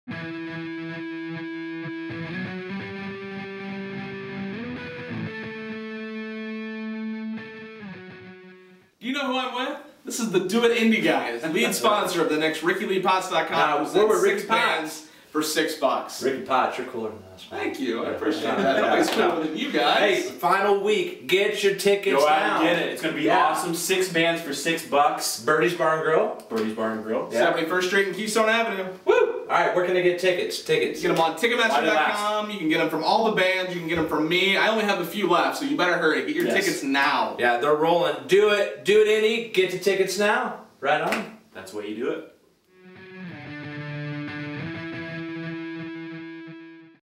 six bands for six bucks - the final countdown
Come out Saturday, August 6th at 7pm- LIVE @ Birdy's Bar & Grill - 71st and Keystone. Big Ben and Tolin will be rocking the night out as event emcee's!